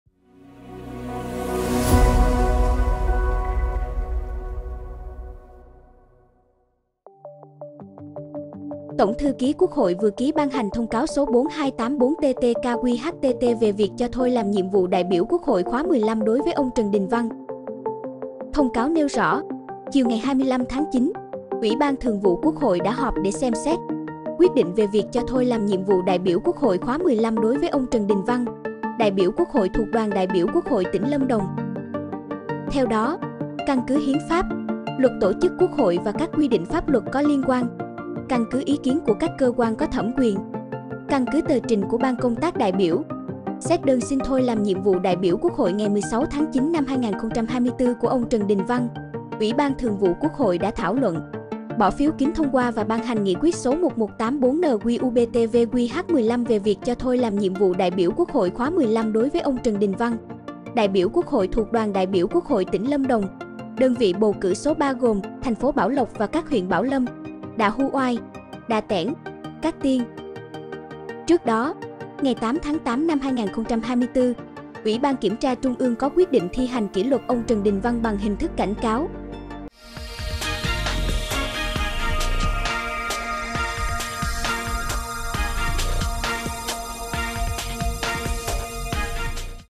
Cho thôi nhiệm vụ đại biểu Quốc hội khóa XV đối với ông Trần Đình Văn
SGGP | Tổng Thư ký Quốc hội vừa ký ban hành thông cáo số 4284/TTKQH-TT về việc cho thôi làm nhiệm vụ đại biểu Quốc hội khóa XV đối với ông Trần Đình Văn.